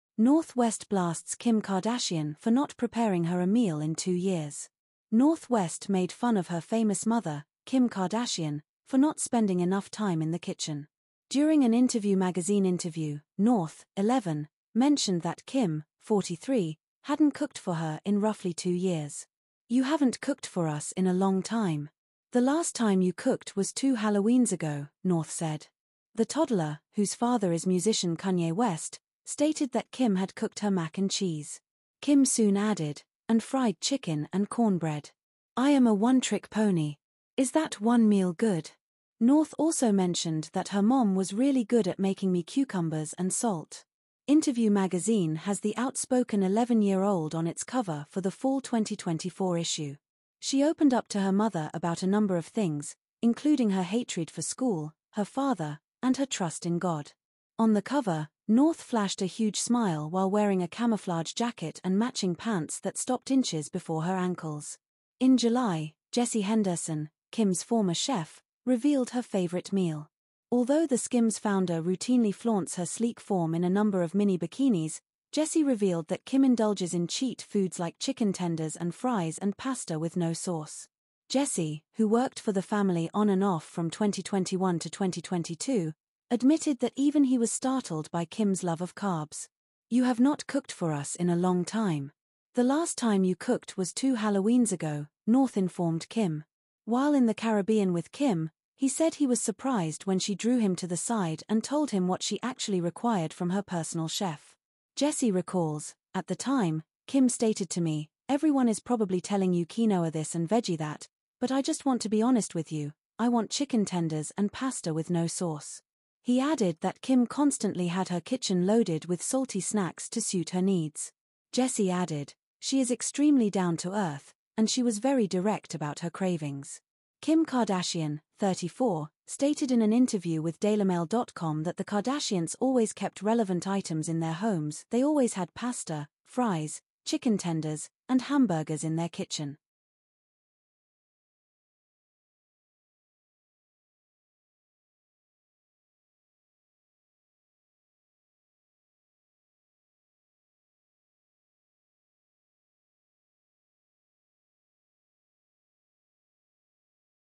North West SLAMS Kim Kardashian: You Haven't COOK For Us In 2 Years
North West blasts Kim Kardashian for not preparing her a meal in two years. North West made fun of her famous mother, Kim Kardashian, for not spending enough time in the kitchen. During an Interview Magazine interview, North, 11, mentioned that Kim, 43, hadn't cooked for her in roughly two years. ‘You haven't cooked for us in a long time. The last time you cooked was two Halloweens ago, North said. The toddler, whose father is musician Kanye West, stated that Kim had cooked her "mac and cheese." Kim soon added: "And fried chicken and cornbread." I am a one-trick pony. Is that one meal good? North also mentioned that her mom was "really good at making me cucumbers and salt." Interview Magazine has the outspoken 11-year-old on its cover for the Fall 2024 issue. She opened up to her mother about a number of things, including her hatred for school, her father, and her trust in God. On the cover, North flashed a huge smile while wearing a camouflage jacket and matching pants that stopped inches before her ankles. In July, Jesse Henderson, Kim's former chef, revealed her favorite meal. Although the Skims founder routinely flaunts her sleek form in a number of mini bikinis, Jesse revealed that Kim indulges in cheat foods like 'chicken tenders and fries' and 'pasta with no sauce. Jesse, who worked for the family on and off from 2021 to 2022, admitted that even he was startled by Kim's love of carbs. You have not cooked for us in a long time.